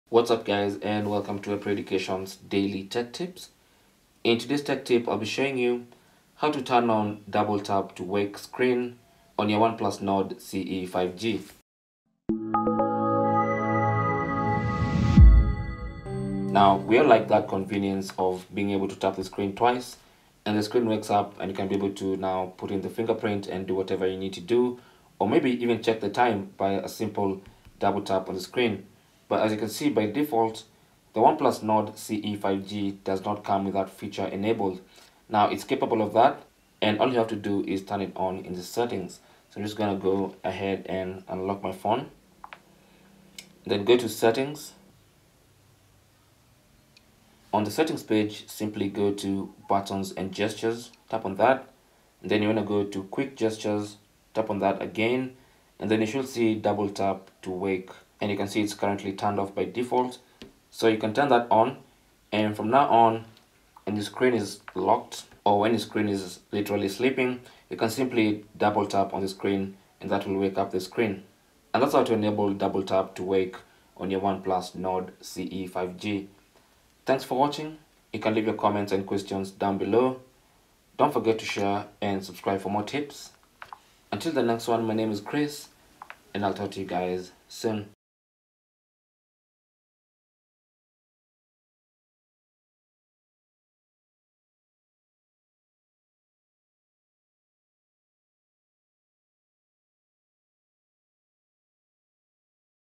OnePlus Nord CE 5G - How To Turn On 'Double Tap' To Wake Screen
The Oneplus Nord CE 5G does not come with the tap to wake screen feature enabled by default. But it does have the feature and here's how to enable it.

Oneplus Nord CE 5G On Amazon